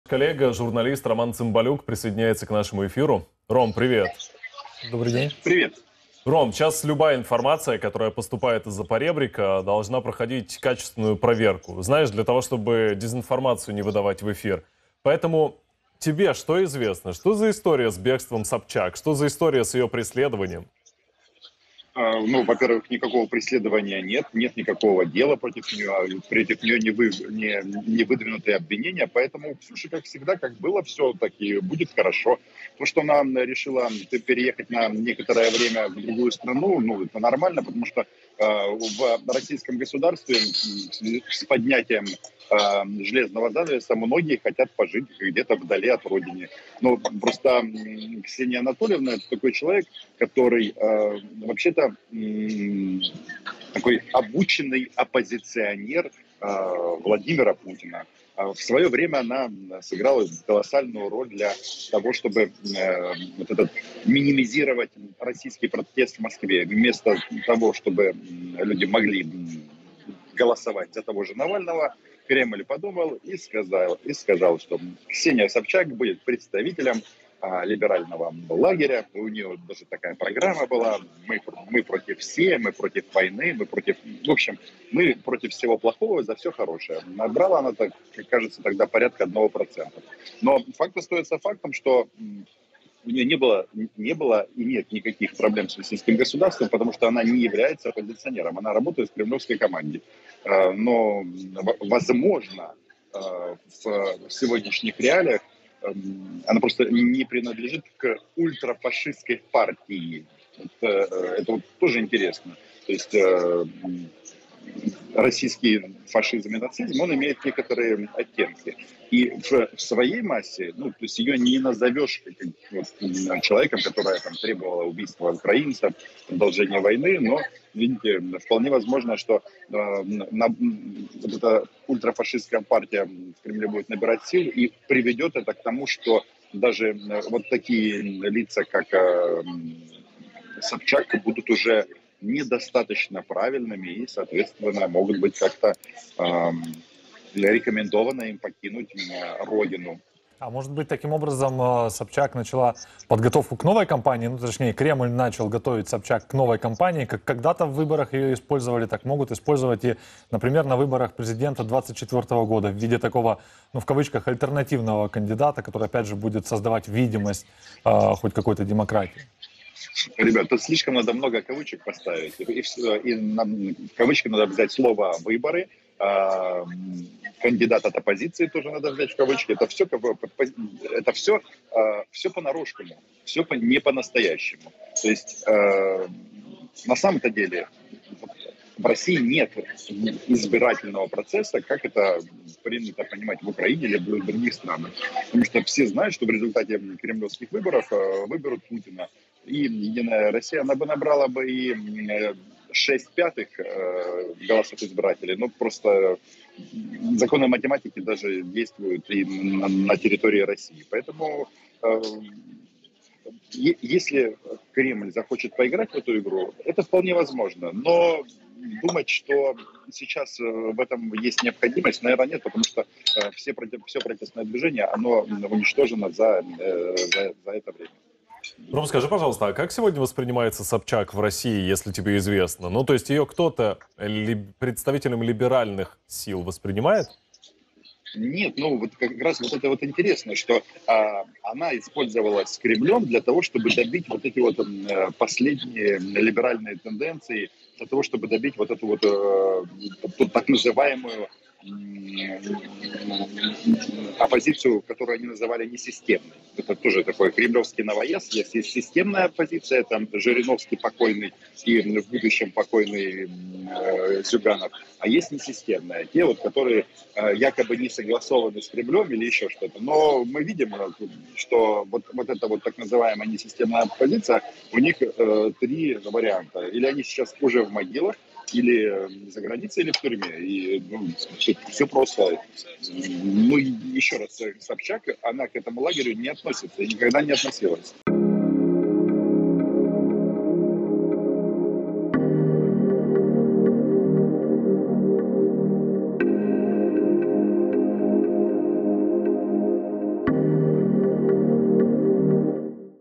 Собчак в бегах. Зачем Путин выгнал "крестницу" из РФ – Цимбалюк
UA - Ксенія Собчак, яку вважають "хрещеницею" Володимира Путіна, втекла з Росії. Це сталося просто так. Жодного тиску на Собчак бути не може, адже вона є особистим навченим опозиціонером кремлівського діда. Подробиці про її роль у Росії розповів журналіст Роман Цимбалюк.

RU - Ксения Собчак, которую считают "крестницей" Владимира Путина, сбежала из России. Это произошло просто так. Никакого давления на Собчак быть не может, ведь она является личным обученным оппозиционером кремлевского деда. Подробности о ее роли в РФ рассказал журналист Роман Цимбалюк.